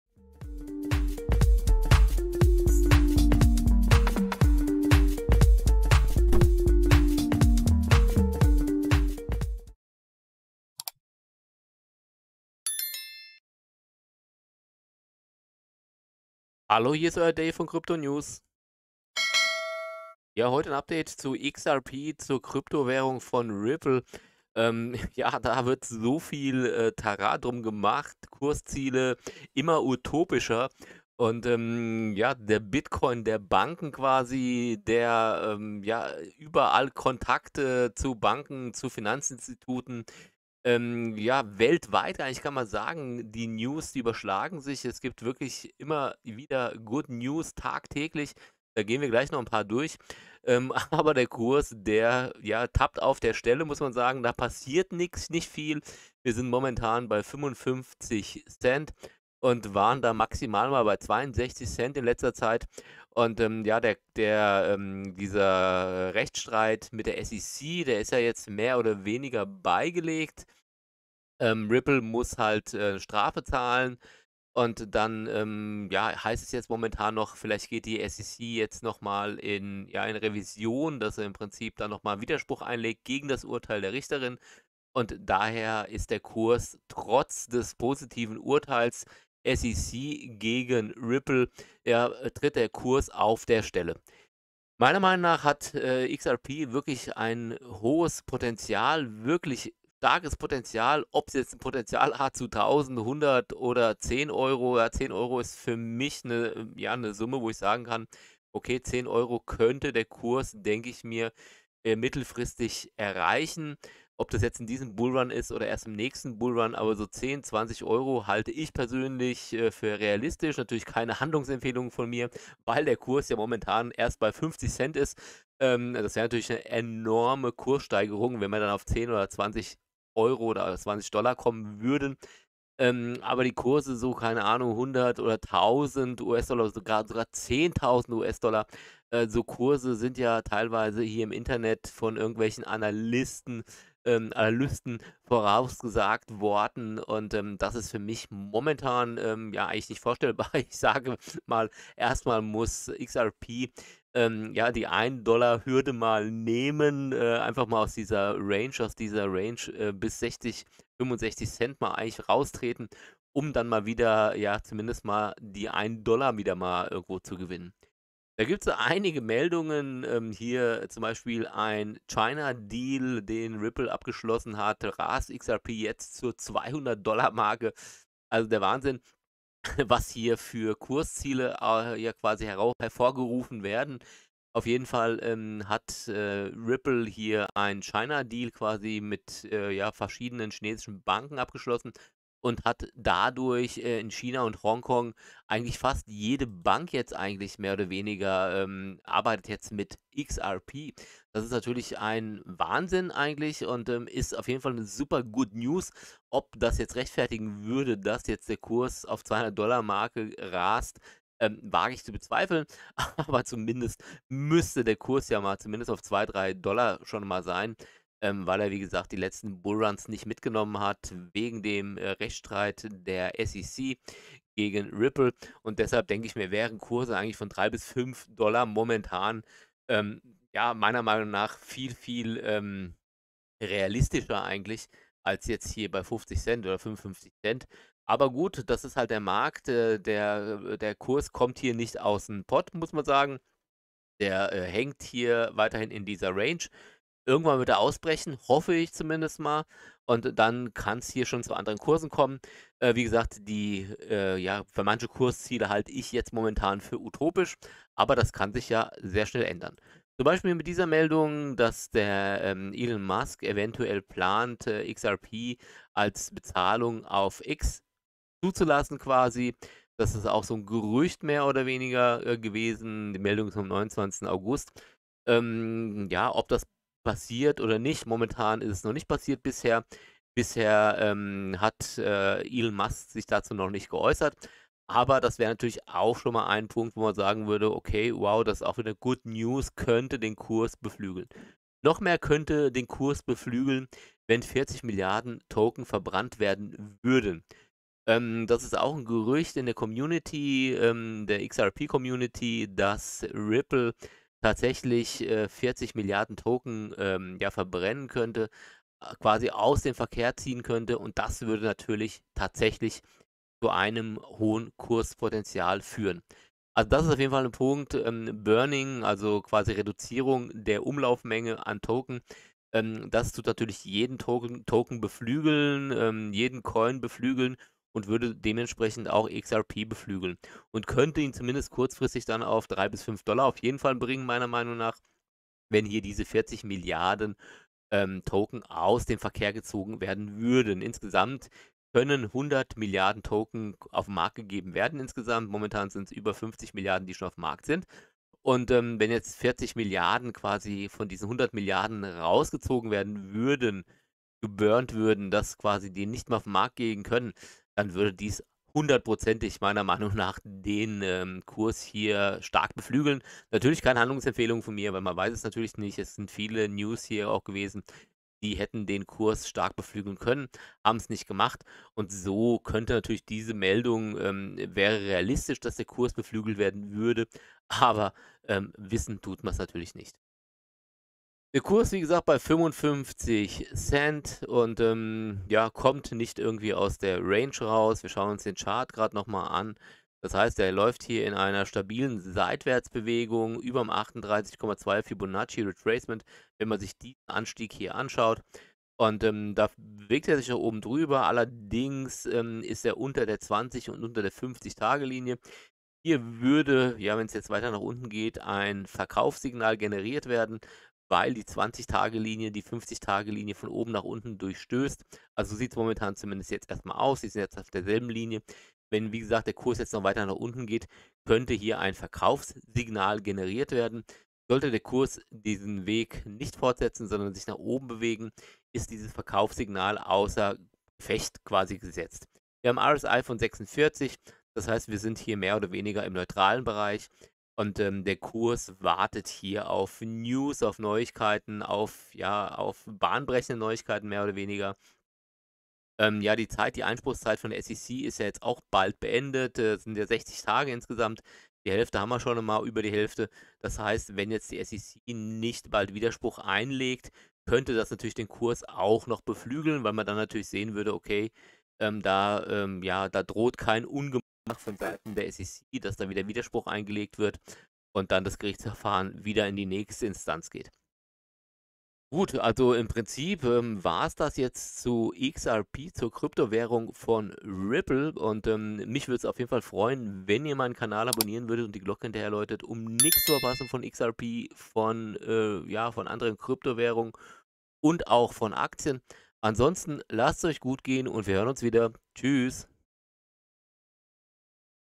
RIPPLE XRP TOKEN COIN UPDATE ✅ WERDEN 40 MRD XRP TOKEN VERBRANNT? ✅ RIPPLE CHINA DEAL ✅ ELON MUSK
KRYPTO NEWS CRYPTO NEWS
Heute wieder ein Update zu XRP von Ripple 🚀🚀🚀. Was gibt es neues zum XRP COIN / TOKEN und wie verhält sich der Kurs von XRP?